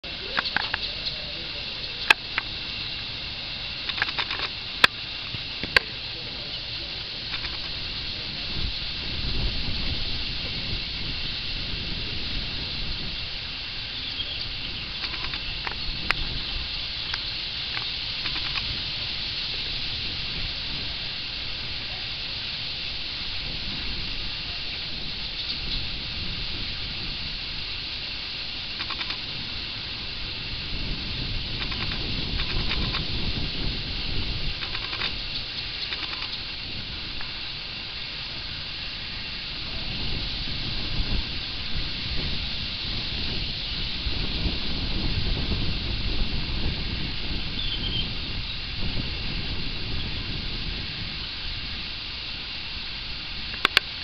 Calamón (Porphyrio porphyrio)
Alimentandose de plantas acuaticas, entre el carrizo del sabogal en el delta del llobregat